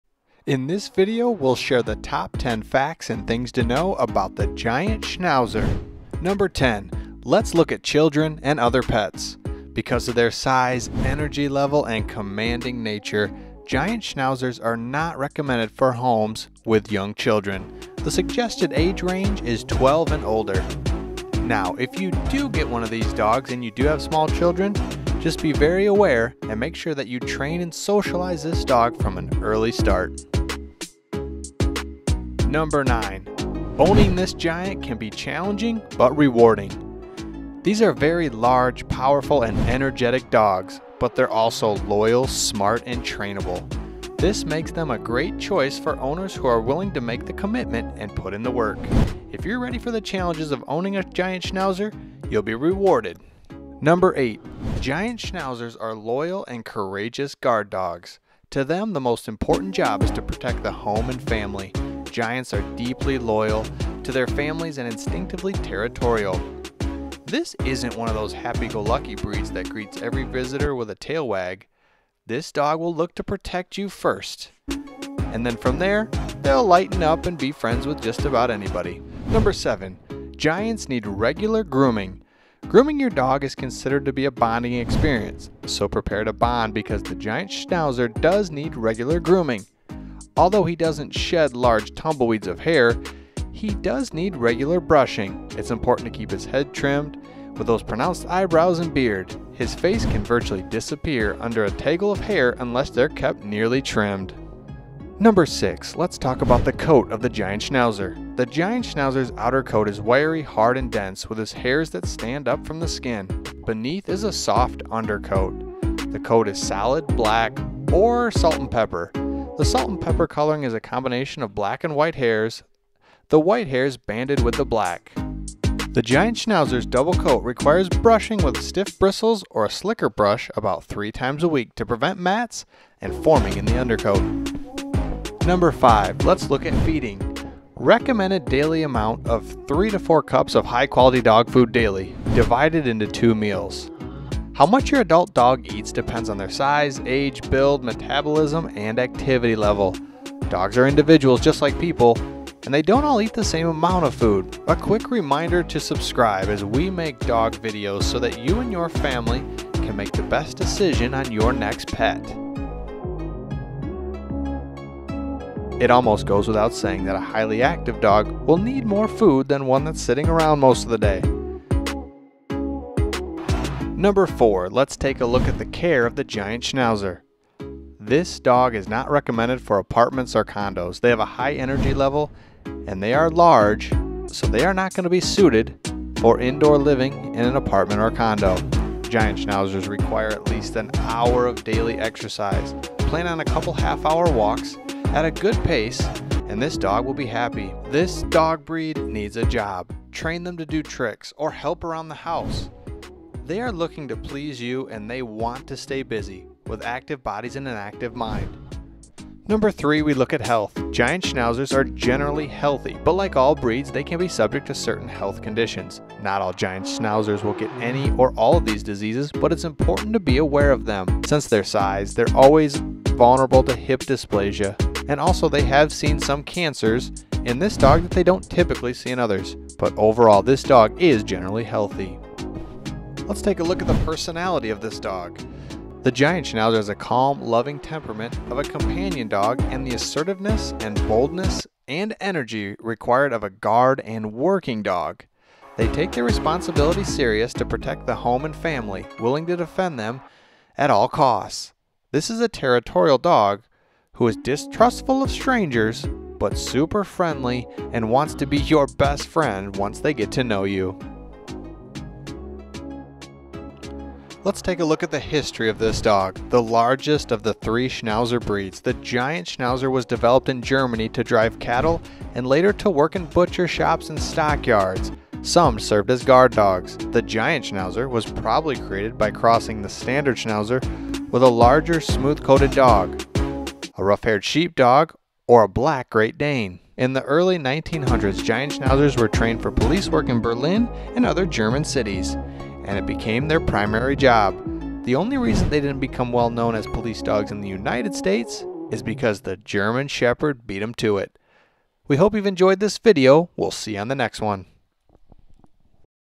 Giant Schnauzer - Top 10 pros and cons of the Giant Schnauzer Dog Breed
Considering a Giant Schnauzer Dog? In this video, we look at what you need to know about the protective, smart, and powerful dog. The Giant Schnauzer is protective, low shedding, and smart. They have active minds and are ready to please.

People often ask:
Is a Giant Schnauzer protective?
Is a Giant Schnauzer good with kids?
Does a Giant Schnauzer shed?

We’ll answer those questions in this video.

We’ll also discuss:
Giant Schnauzer Temperament
Giant Schnauzer Size
Giant Schnauzer Training
Giant Schnauzer Grooming
Giant Schnauzer family?